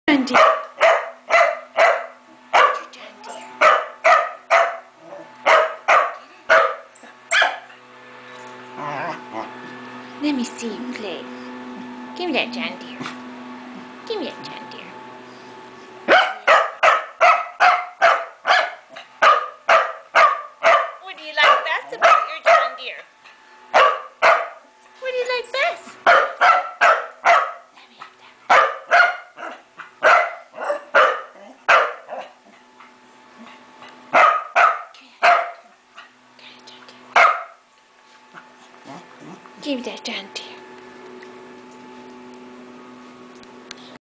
Emma Talk: John Deere Tractors
What I like to do when I have an extra long weekend and a bit of time on my hands... film, of course, my furr children doing what they do best... acting too cute for me to totally capture except through LIVE footage! Apologies for the occasionally blurred footage but wanted to share Emma talking about her John Deere, which was prompted from a FB conversation with a friend of mine.